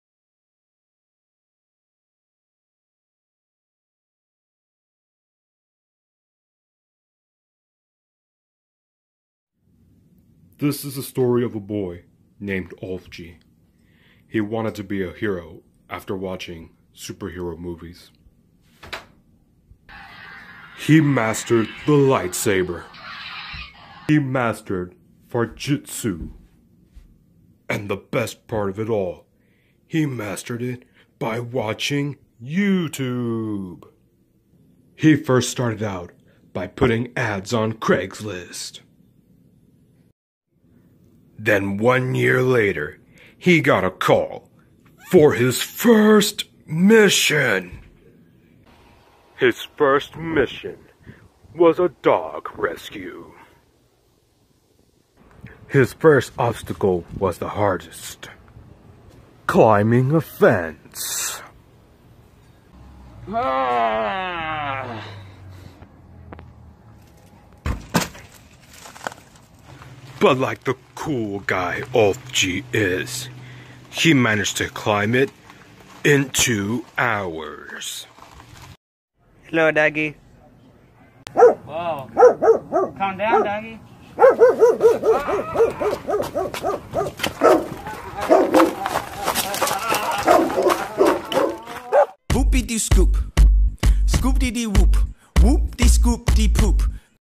OLF-G The Beginning - Epik Film
this is a video i made in 2018 for an idea i had where GFlo wants to be a superhero.

I decided to reupload this video because i wanted to give my ideas of making superhero films another chance.